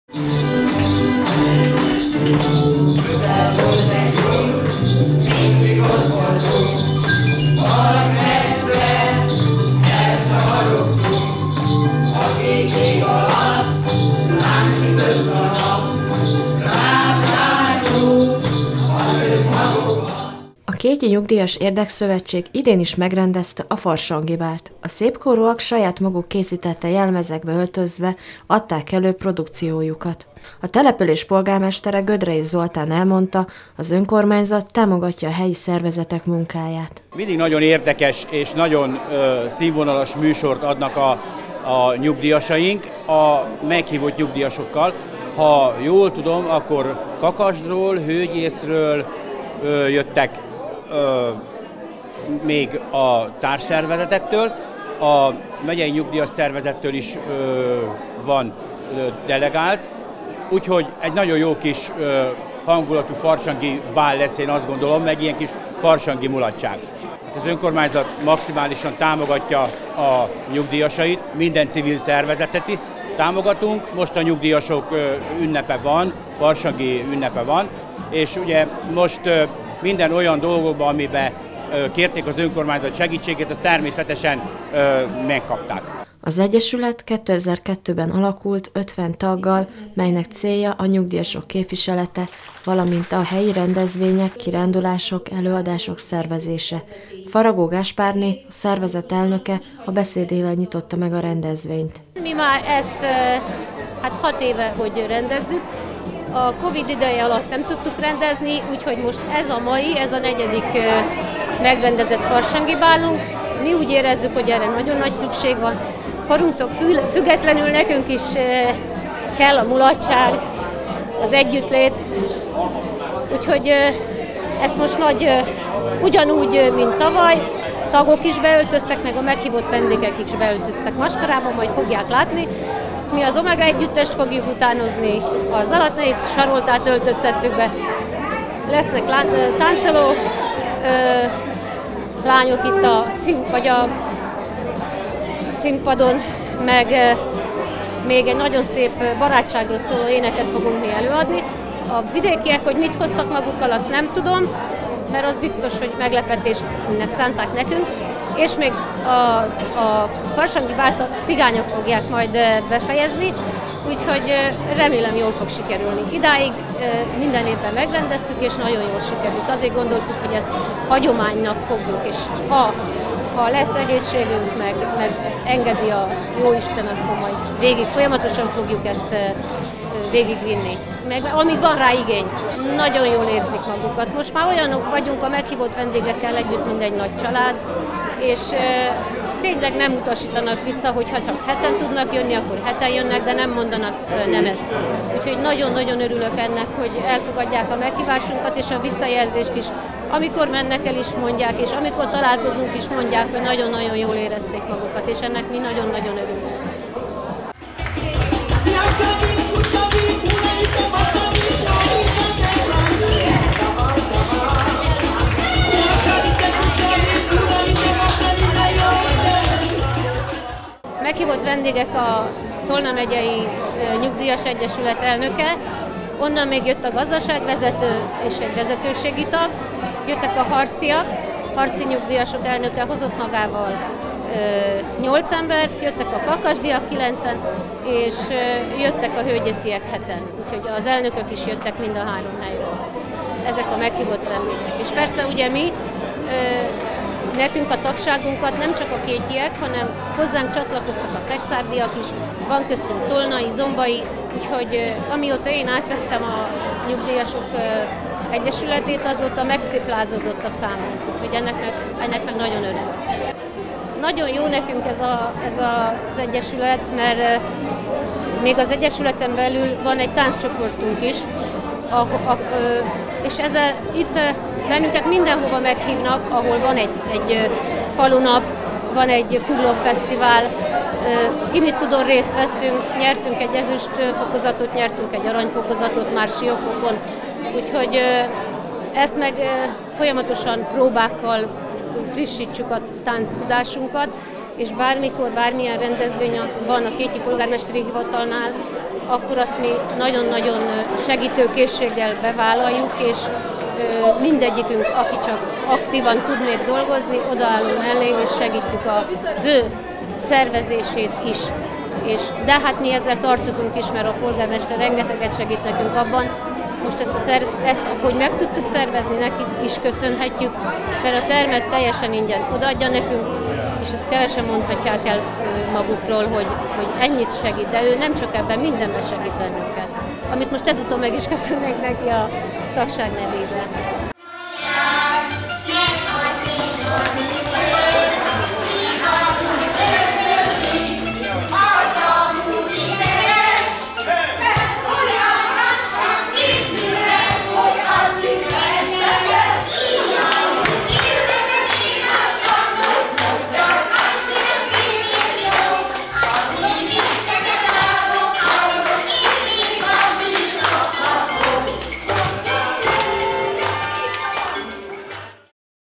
Kétyi nyugdíjasok - Vidám hangulatban telt a farsangi mulatság
A kétyi nyugdíjasoknak farsangi mulatságot rendeztek a község művelődési házában. A rendezvényre, Harc, Kakasd és Hőgyész nyugdíjasai is meghívást kaptak. Zenés, táncos műsort adtak elő a résztvevőknek.